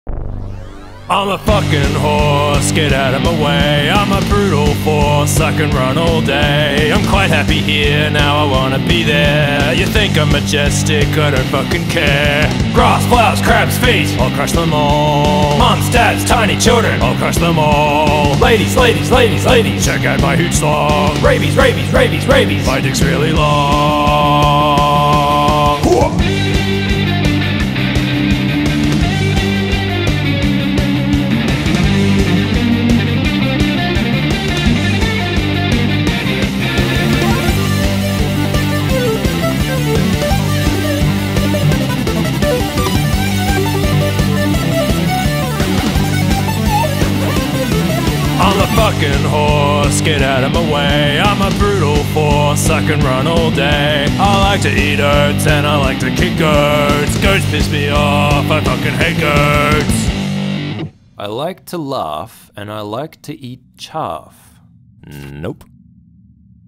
I'M A HORSE
A studly horse dominates and destroys!  I started this one back in February '18 and worked on it when I could. I can't remember why I wanted to do it.

2 shots animated by Jae55555